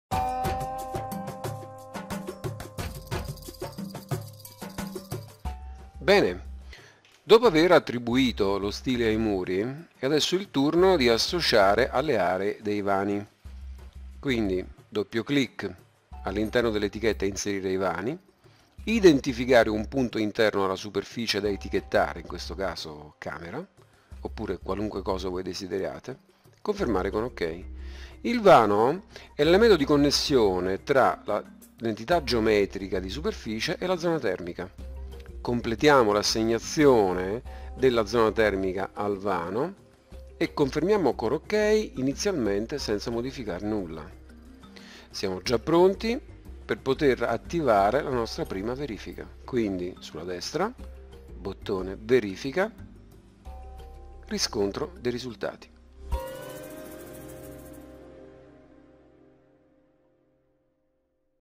Tutorial TermiPlan: Associare le zone termiche ai Vani
- Tutorial TermiPlan: Associare le zone termiche ai Vani. TermiPlan, Software Certificazione Energetica by Analist Group